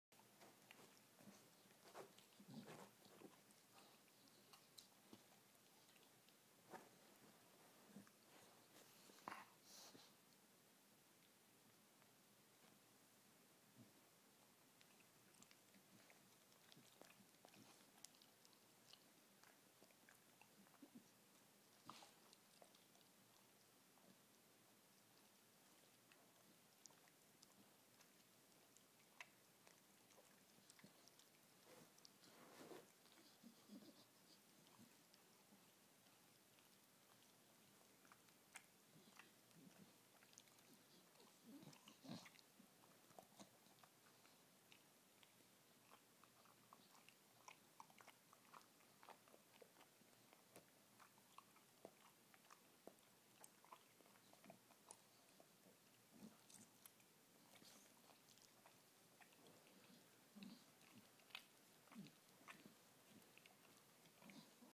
Staffordshire Bull Terrier Kaya verzort Tess
Ook honden moet wel eens tanden poetsen.